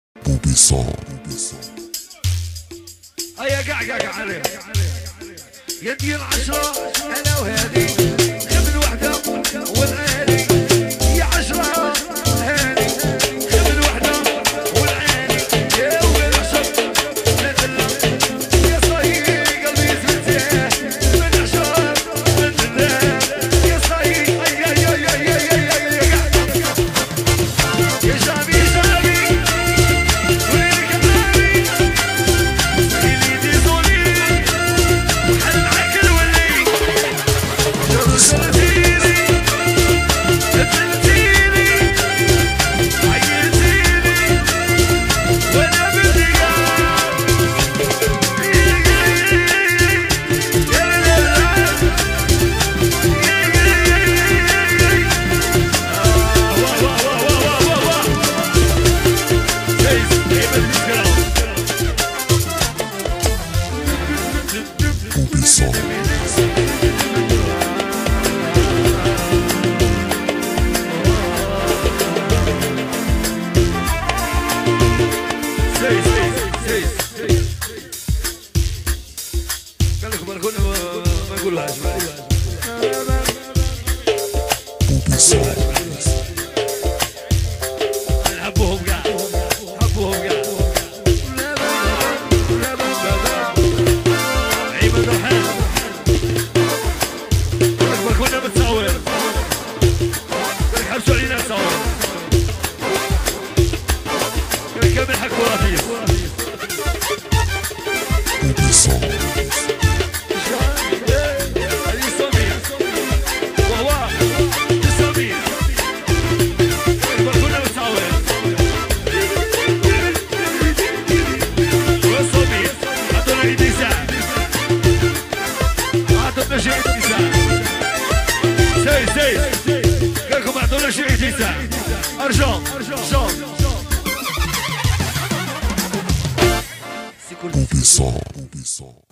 Cheb Djalil et chehab chbab 21 mariage Souhil amouri LIVE BISKRA BOUBI SON By •••►DJ HASNI
LIVE BISKRA BOUBI SON
      Mariage Souhil amouri
           Studio  Boubi Son

▶️ Cheb Djalil
▶️ Pianiste :CHIHAB CHBAB
▶️ Perc : Azza Casa
▶️ Mix : Boubi Hasni
▶️ Distributeur Digital : Sido SL